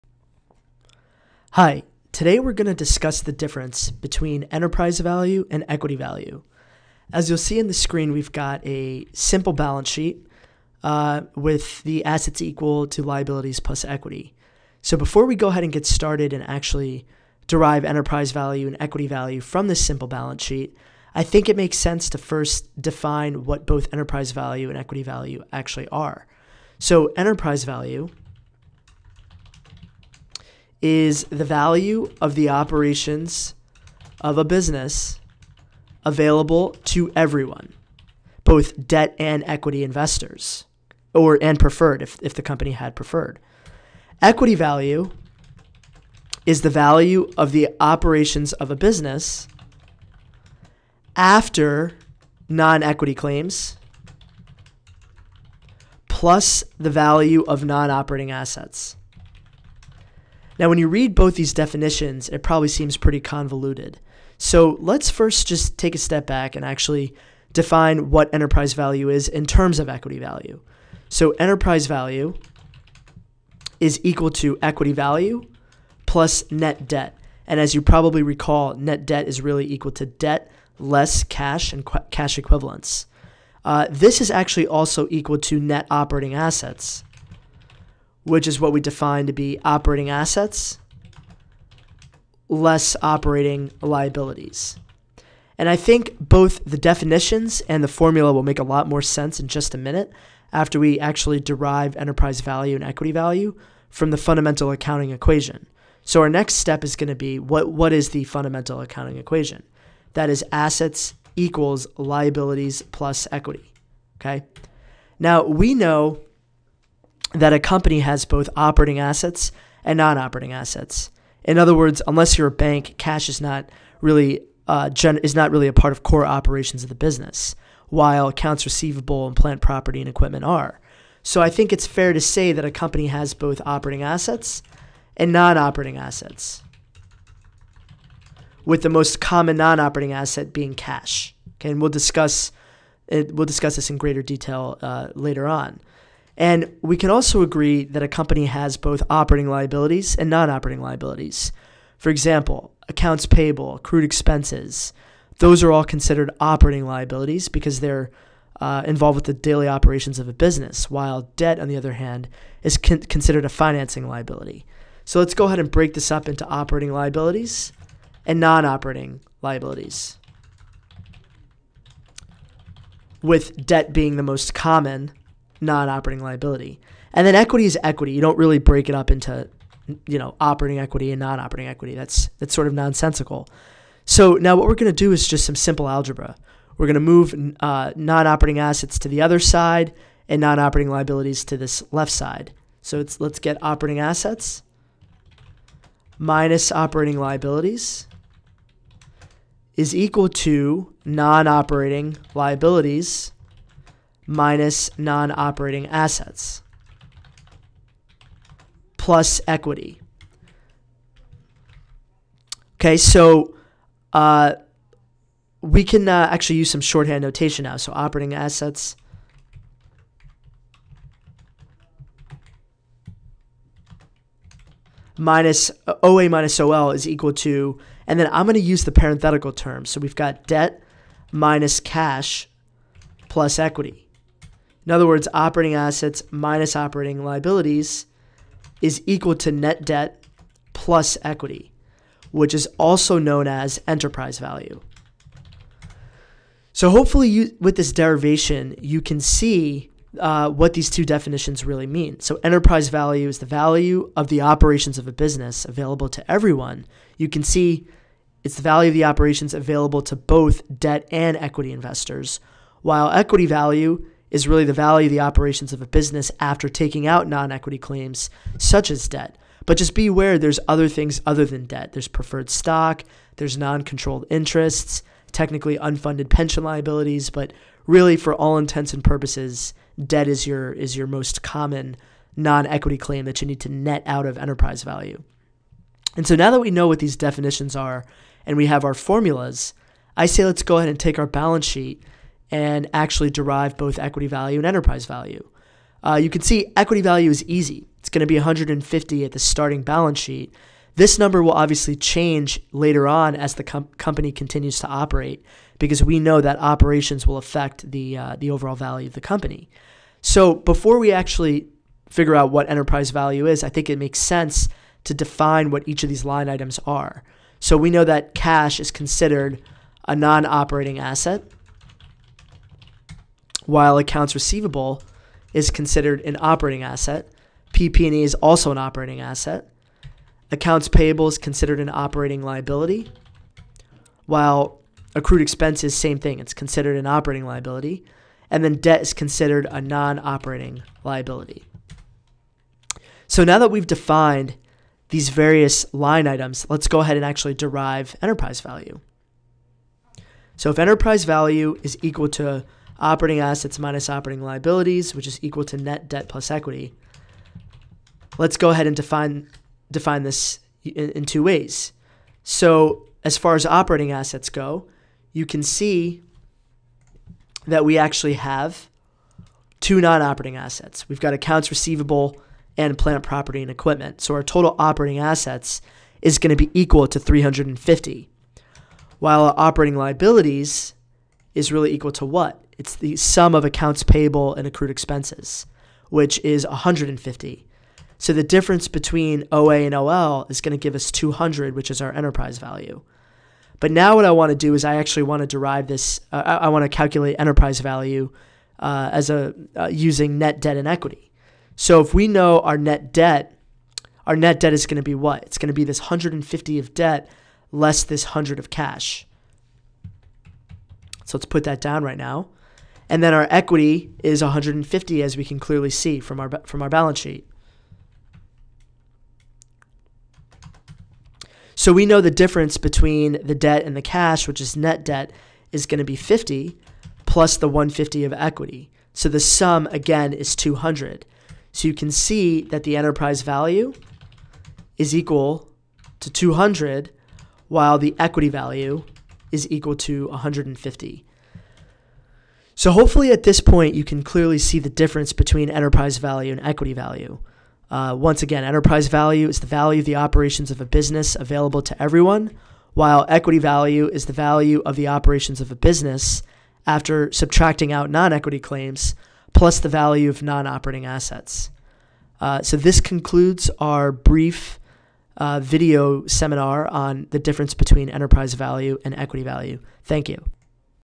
Financial Modeling Quick Lesson: Enterprise Value vs Equity Value
In this lesson, we deconstruct the often-misunderstood definition of enterprise value by showing that it is simply a re-ordering of the fundamental accounting equation 'Assets = Liabilities + Equity'.

For more about enterprise value and other financial modeling topics, go